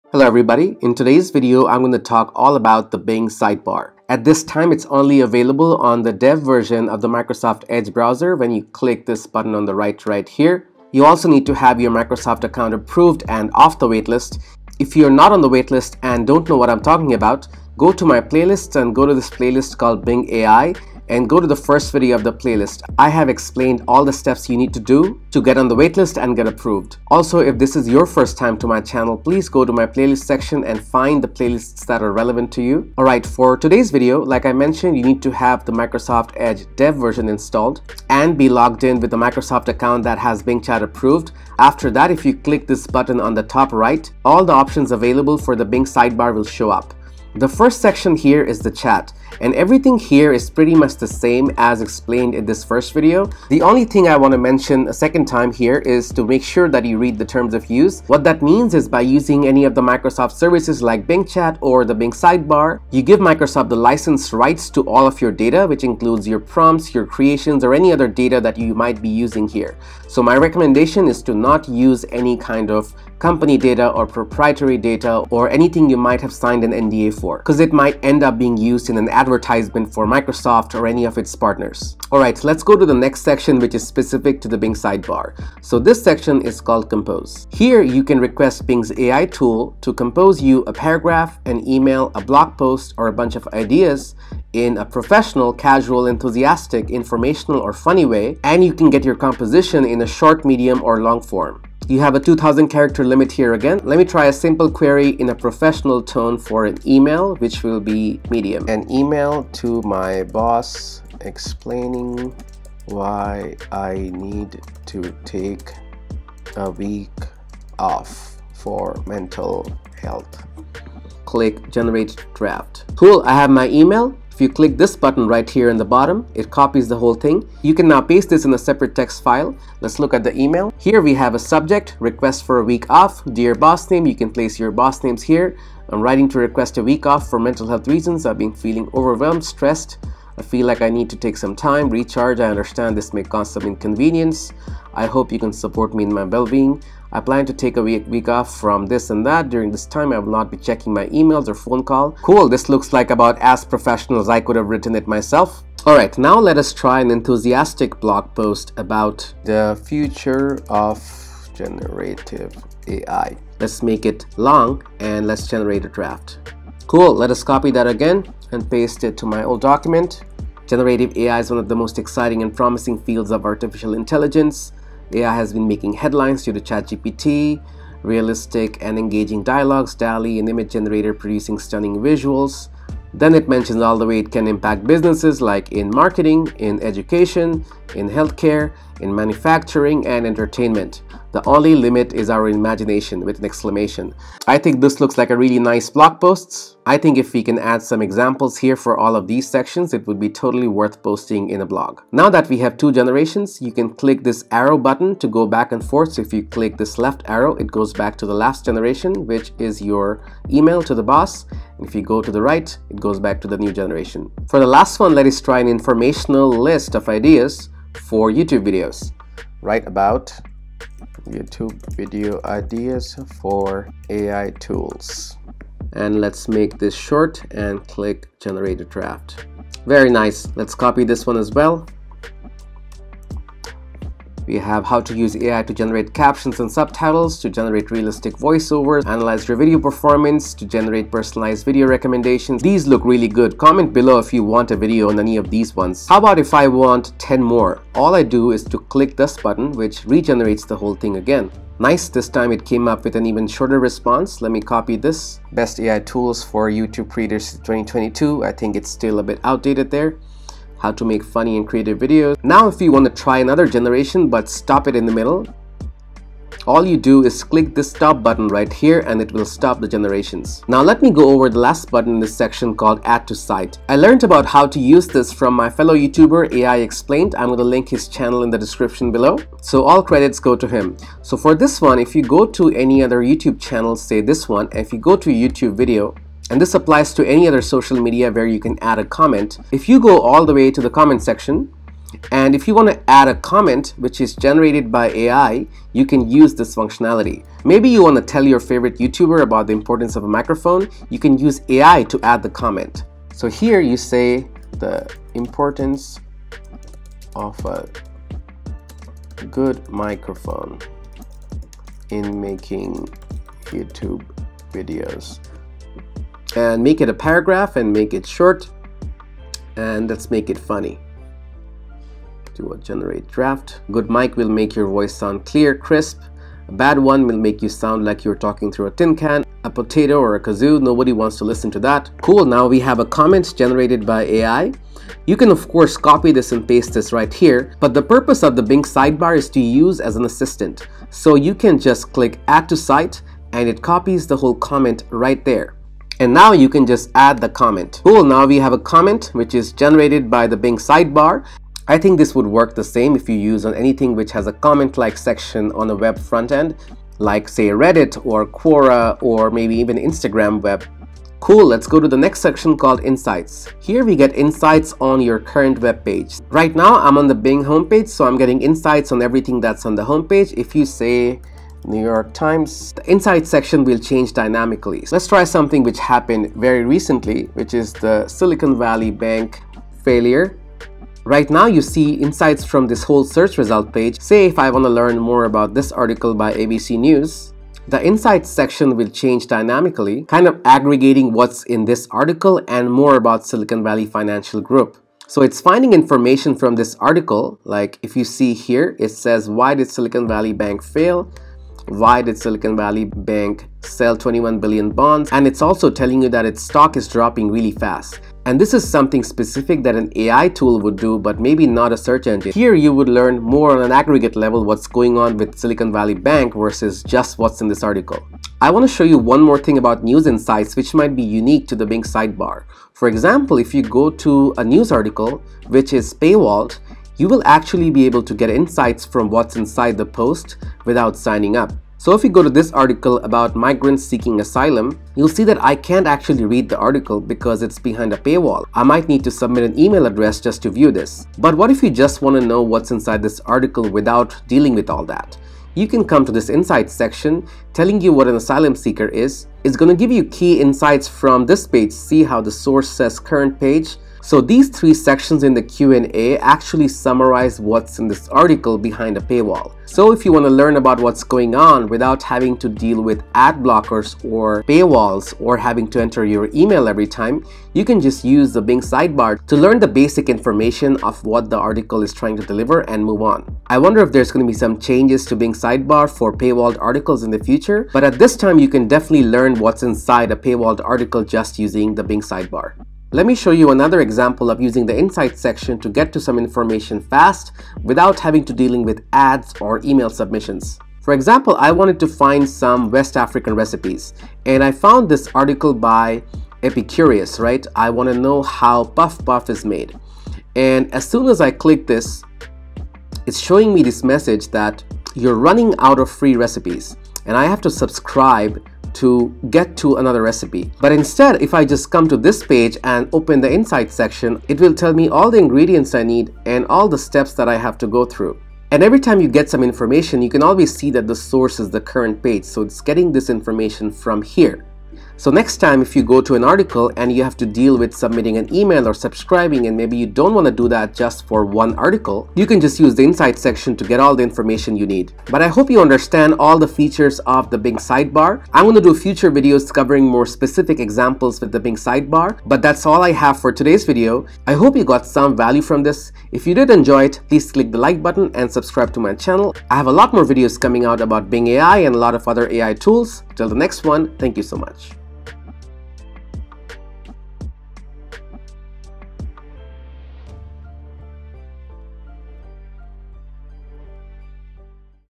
Microsoft bing sidebar | bing chatgpt - beginner's tutorial (Part 2)
- In this bing ai tutorial, I go over the microsoft bing sidebar. I will show you how to use the bing chatbot via the sidebar version of bing chatgpt.

Links from today’s video :
➜  Microsoft edge dev version download

Timestamps :
➜ 0:43 Bing ai terms of use overview
➜ 1:44  Compose email
➜ 2:36 Compose blog post
➜ 3:34 Compose ideas
➜ 4:31 Add to site
➜ 6:01  Insights
➜ 6:58 Insights hack